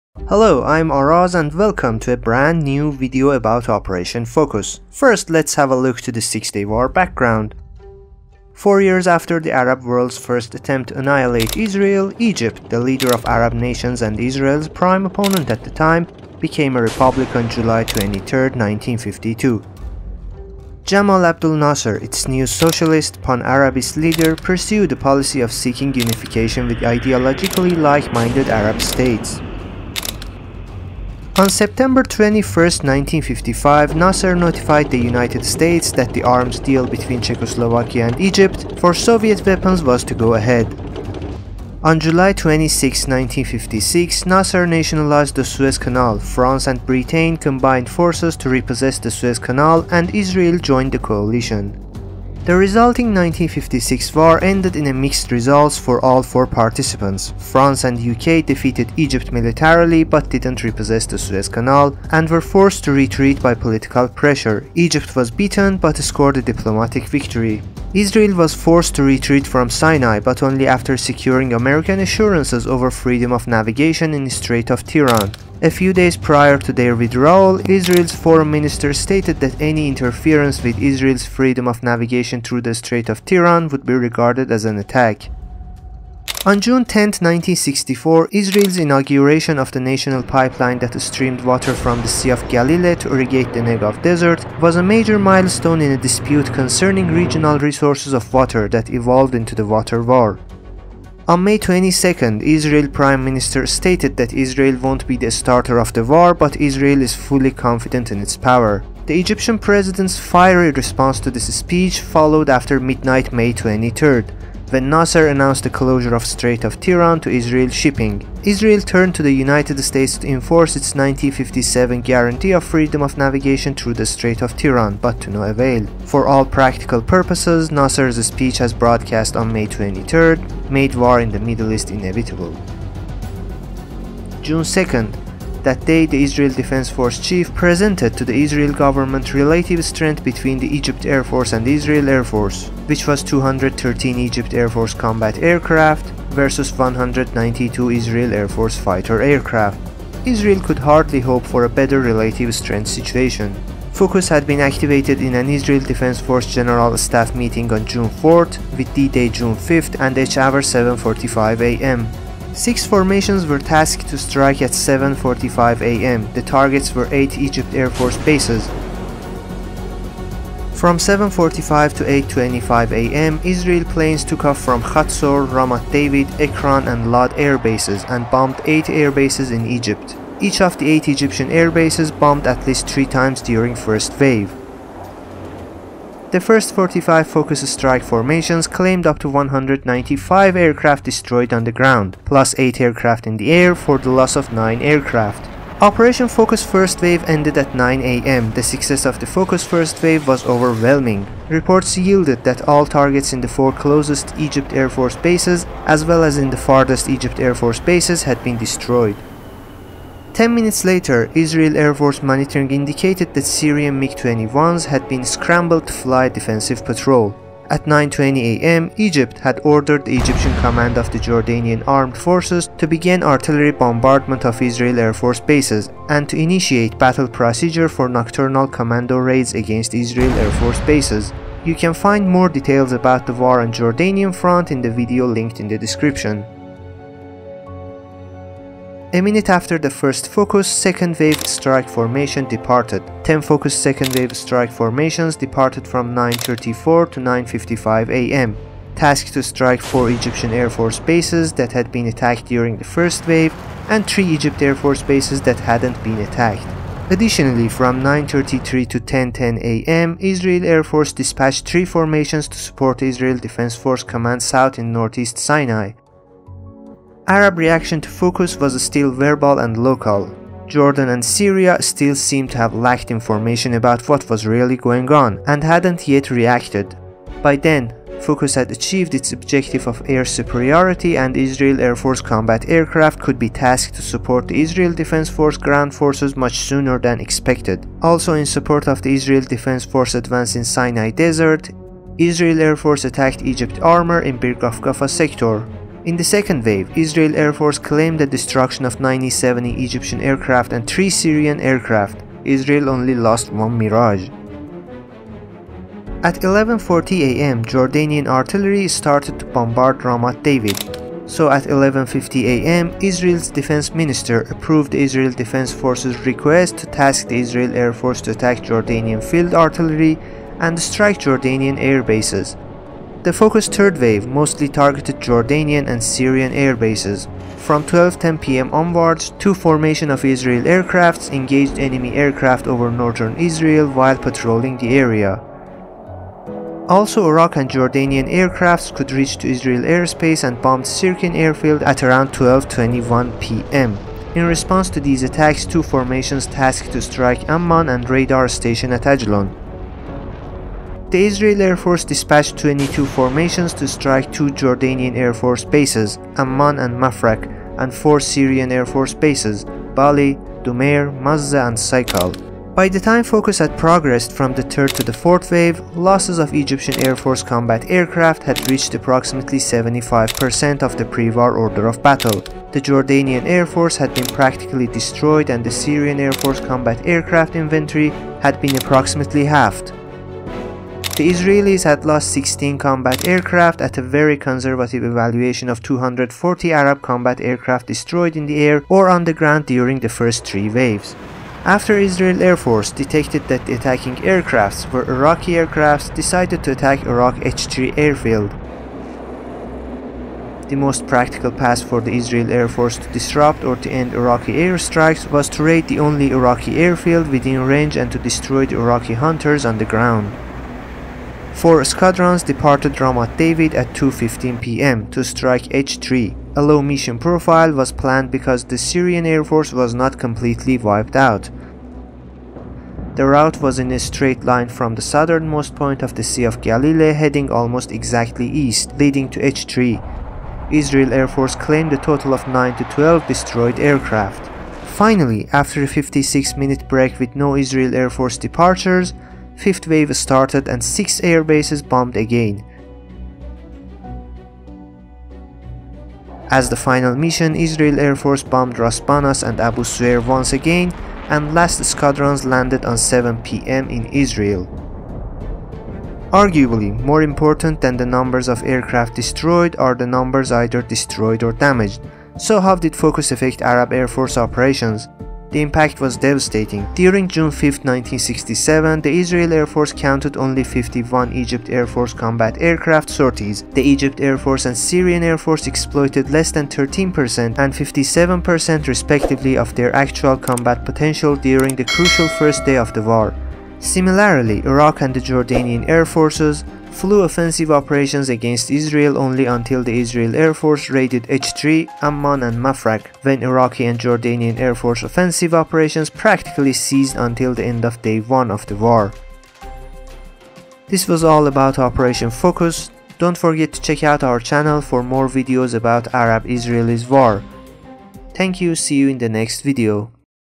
Operation Focus 1967 | Arabs - Israeli Six-Day War | Operation Moked
Operation Focus was the start of Israeli airstrikes against Egypt during the six-day war 1967. on June4, 1967, Operation focus activated to destroy the Egyptian airforce on the ground. on June5, 1967 at 7:45 AM, the first formations departed Israel and bombed 8 airbases in Egypt. Operation Moked ( Operation Focus ) was one the most successful operations in air battles history. Operation focus in the second Arab-Israeli war reduced Egypt's air power to 13 percent and brought air superiority for Israel during the six-day war of 1967.
Arguably, more important than the numbers of aircraft destroyed are the numbers either destroyed or damaged. So how did Focus affect Arab Airforce operations? The impact was devastating. During June 5, 1967, the IAF counted only 51 EAF combat aircraft sorties. The EAF and SAF exploited less than 13 percent and 57 percent respectively of their actual combat potential during the crucial first day of the war,
0:00 intro
2:14 First Wave
3:56 Second Wave
5:23 Third Wave
7:37 Casualties